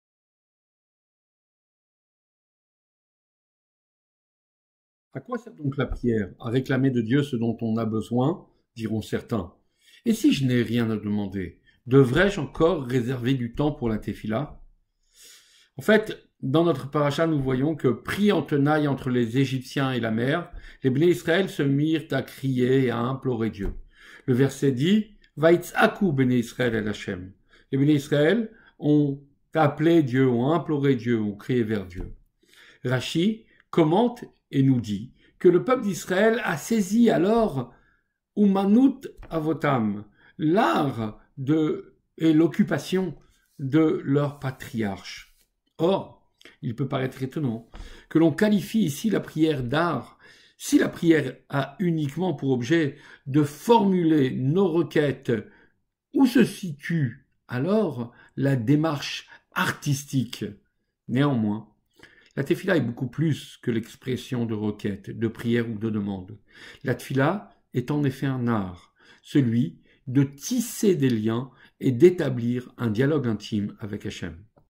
Vivre avec son Temps - Paracha : A Quoi Sert La Prière
À quoi sert donc la prière ? À réclamer de D.ieu ce dont on a besoin, diront certains. Et si je n’ai rien à demander, devrais-je encore réserver du temps pour la Téfila ?
Pris en tenailles entre les Égyptiens et la mer, les Béné-Israël se mirent à crier et à implorer D.ieu. Le verset dit : « Vayitsakou Béné-Israël El Hachem. » Rachi commente et nous dit que le peuple d’Israël a saisi alors l’art et l’occupation de leurs Patriarches.
Or, il peut paraitre étonnant que l’on qualifie ici la prière d’art. Si la prière a uniquement pour objet de formuler nos requêtes, où se situe alors la démarche artistique ?
Néanmoins, la Téfila est beaucoup plus que l’expression de requêtes, de prières ou de demandes. La Téfila est en effet un art, celui de tisser des liens et d’établir un dialogue intime avec Hachem.

Vivre avec son Temps – Paracha

Pensée Quotidienne inspirée des enseignements du Rabbi de Loubavitch

Proposée par Rav Eliahou DAHAN, Lille